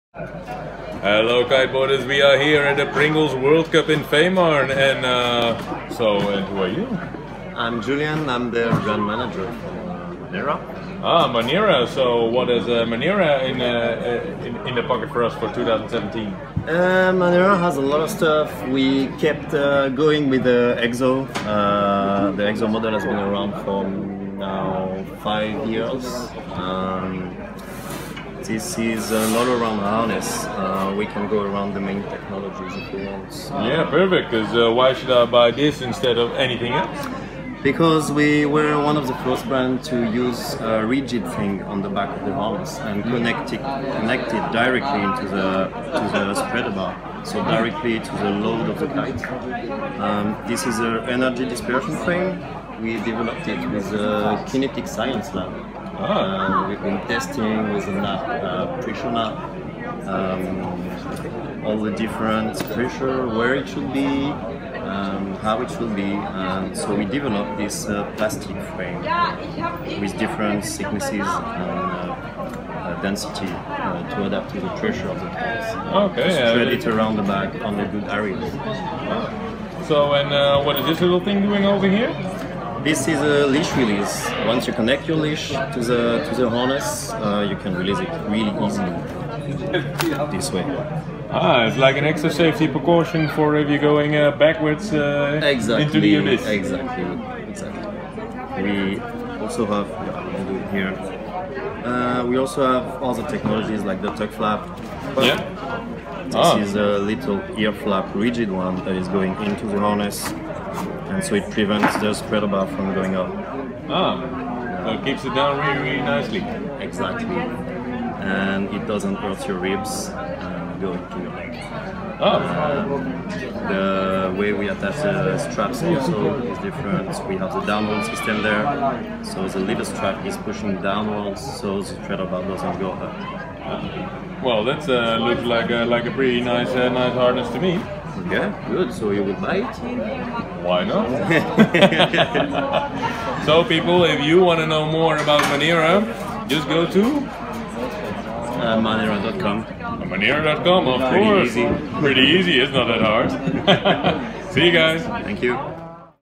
Manera Exo kite Harness
At the 2016  Pringles World Cup on Fehmarn we meet with Julian. The Brand manager of Manera. He explains us why we should try & buy a Manera kitesurfing harness.
The ergonamic frame for your back is one of the big features of the Eo harness.